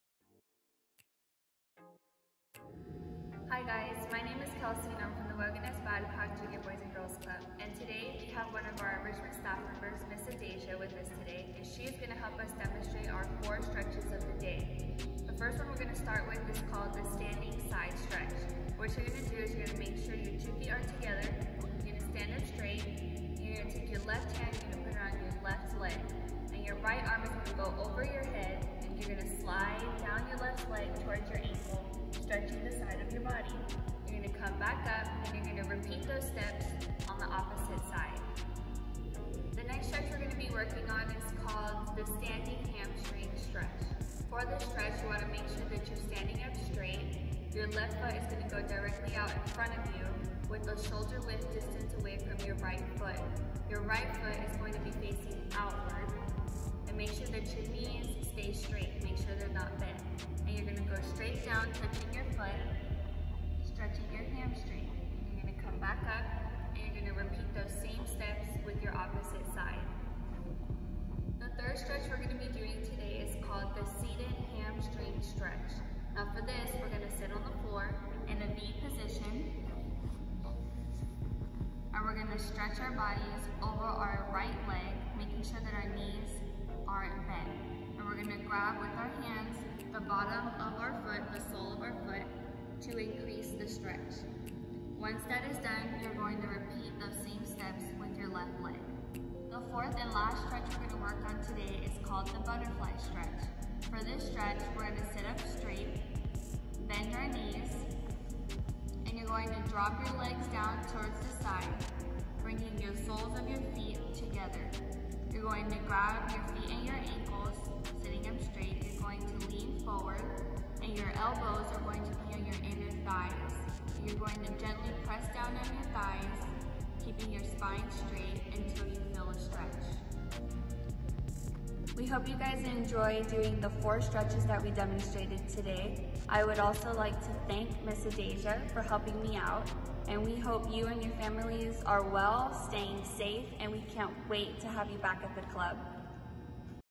Health & Fitness: Stretches of the Day
Join Ms. Kelsy and Ms. Adaejah in these stretches to get your body moving!